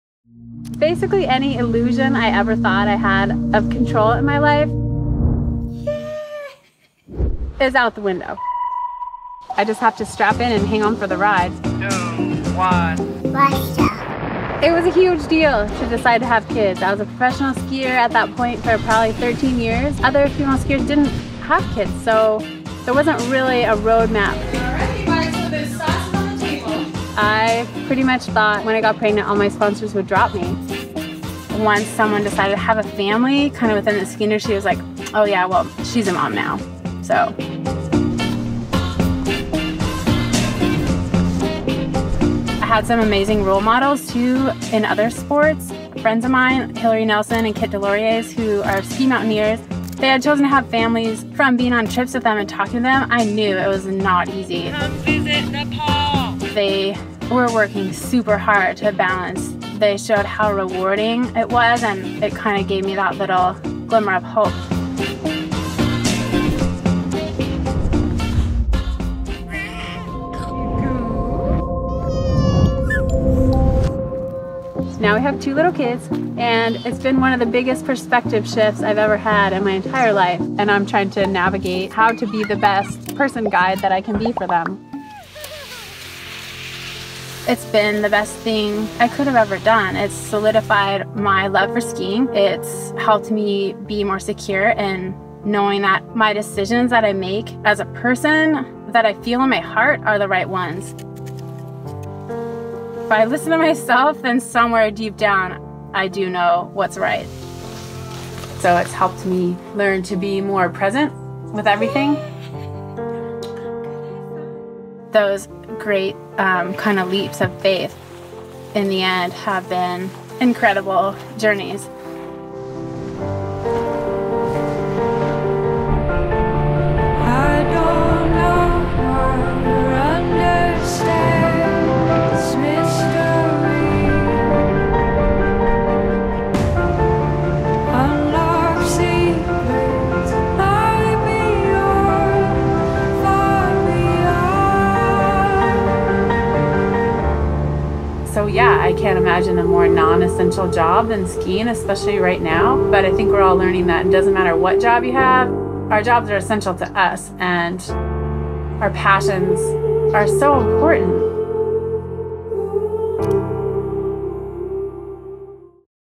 PERSPECTIVES: Ingrid Backstrom | The North Face
Are you a mother or a skier? Professional skier Ingrid Backstrom struggled with this question when having kids and balancing a career. By pursuing both, she learned that it is only you who can define who you are.